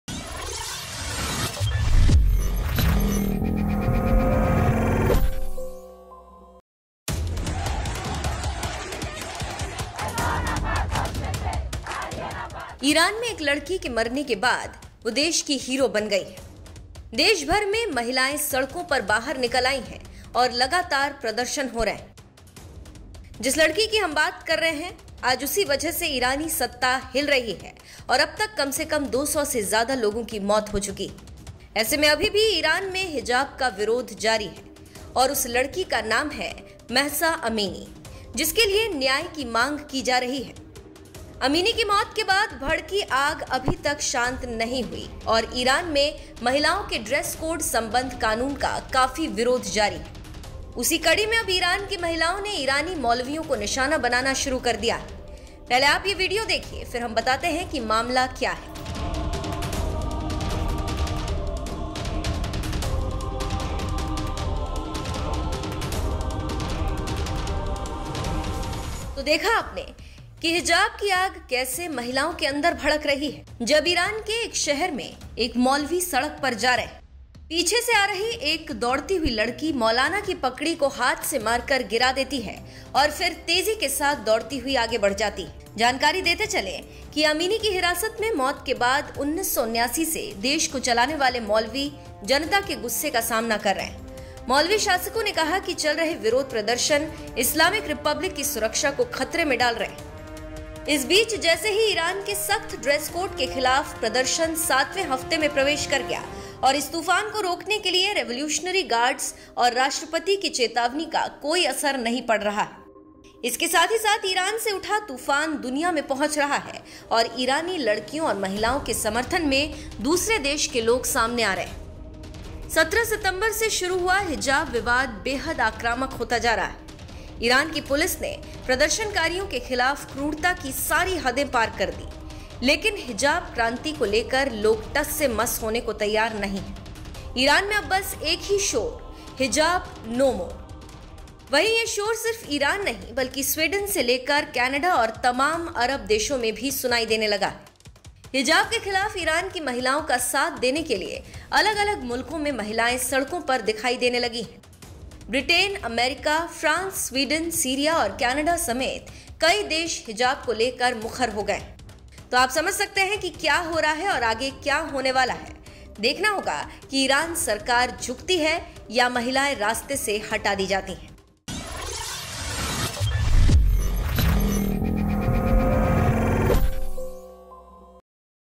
ईरान में अब मौलवियों की उतारी जा रही पगड़ी, वीडियो देख रूह कांप जाएगा !
ईरान में हिजाब के खिलाफ जारी विरोध प्रदर्शन के बीच हाल के दिनों में सोशल मीडिया में कई वीडियो वायरल हो रहे हैं… अलग-अलग जगहों पर ईरानियों के द्वारा मौलवियों की पगड़ी उतारने के कई वीडियो शेयर किए जा रहे हैं…

amin
 rishabh